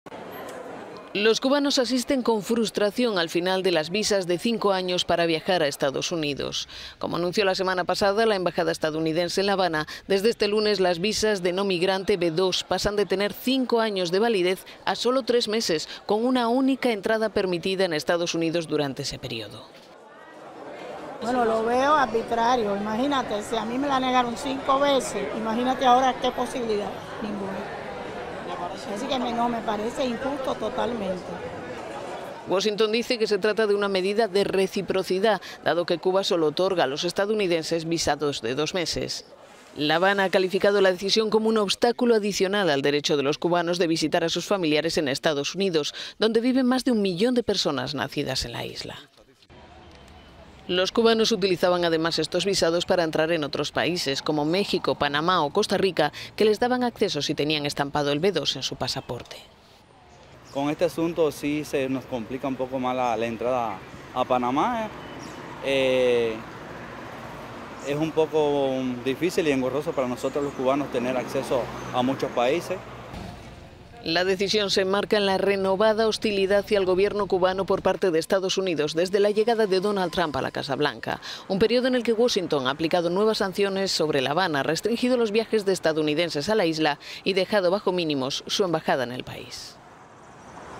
EEUU elimina los visados de cinco años para los cubanos
EEUU elimina la visa de cinco años para los cubanos. Las llamadas visas B2, que duraban cinco años y los cubanos utilizaban para visitar a sus familias, pasan a tener este lunes solo tres meses de validez. …